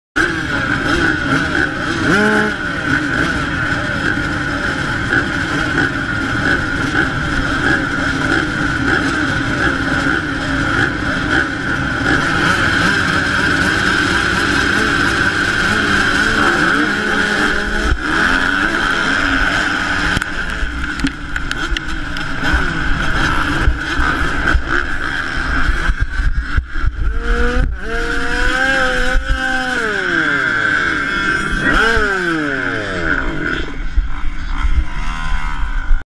Whiteway Barton Crash
Getting beaten up before the first corner